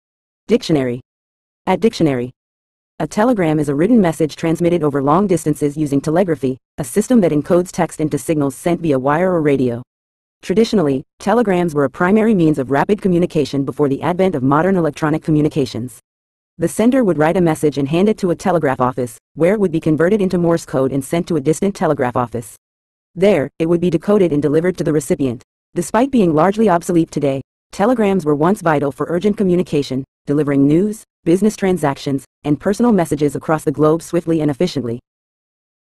Telegram Meaning In English
A telegram is a written message transmitted over long distances using telegraphy, a system that encodes text into signals sent via wire or radio. Traditionally, telegrams were a primary means of rapid communication before the advent of modern electronic communications. The sender would write a message and hand it to a telegraph office, where it would be converted into Morse code and sent to a distant telegraph office. There, it would be decoded and delivered to the recipient. Despite being largely obsolete today, telegrams were once vital for urgent communication, delivering news, business transactions, and personal messages across the globe swiftly and efficiently.